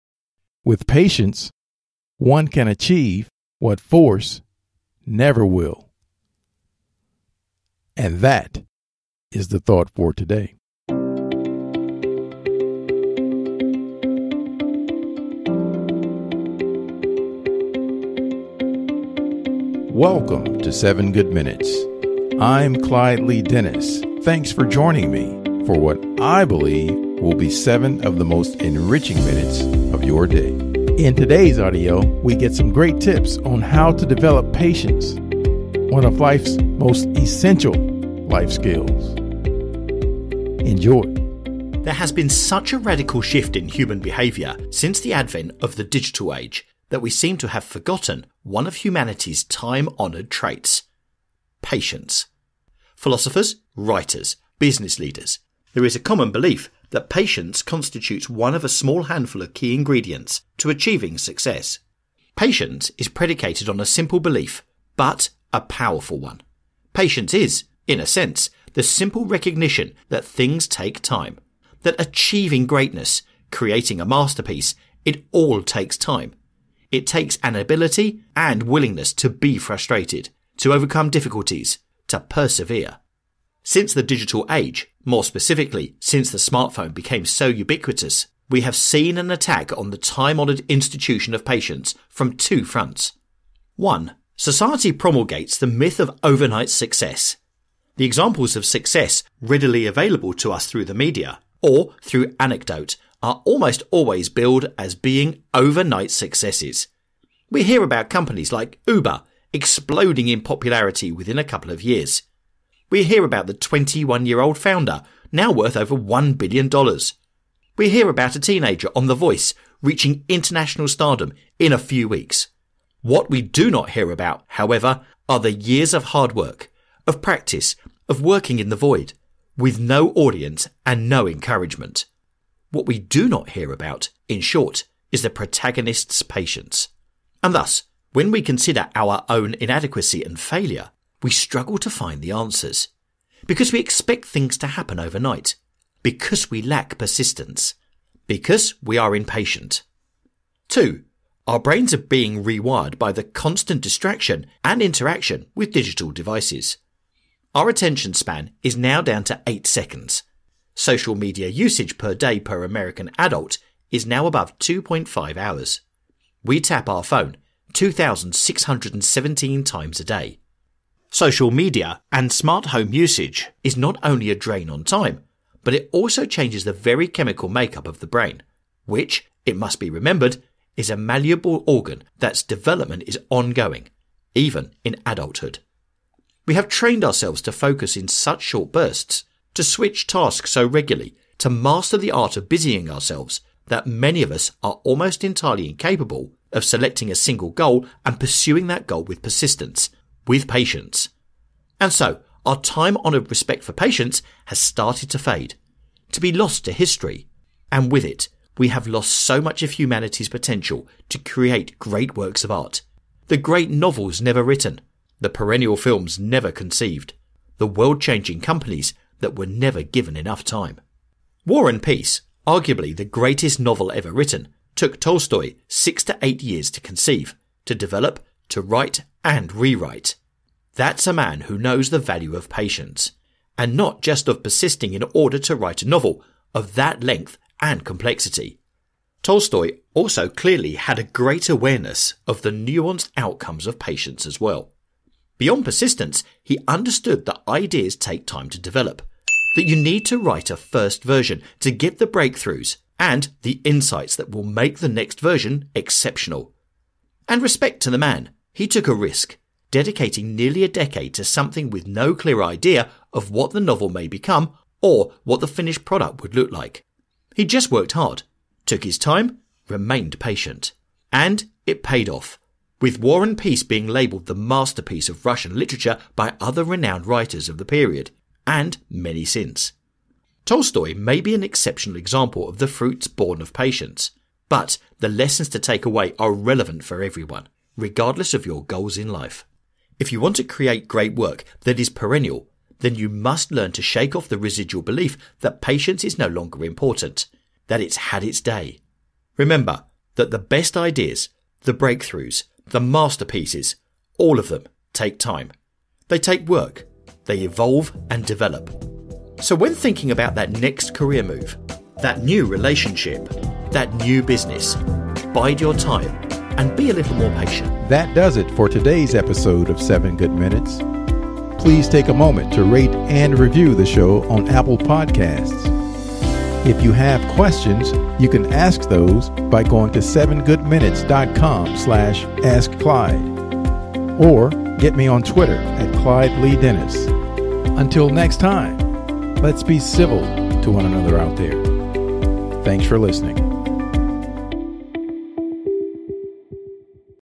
Essential Life Skills: How To Develop Patience
In this episode of the 7 Good Minutes Podcast, we get some great tips on how to develop patience. One of life's most essential life skills.

Audio clip courtesy of our friends at The Art of Improvement

How To Develop Patience

► Getting Over Mistakes Can Be Hard Here’s How To Make It Easier

► Here’s A Simple Goal Setting Guide That Will Help Make Goal Setting Easier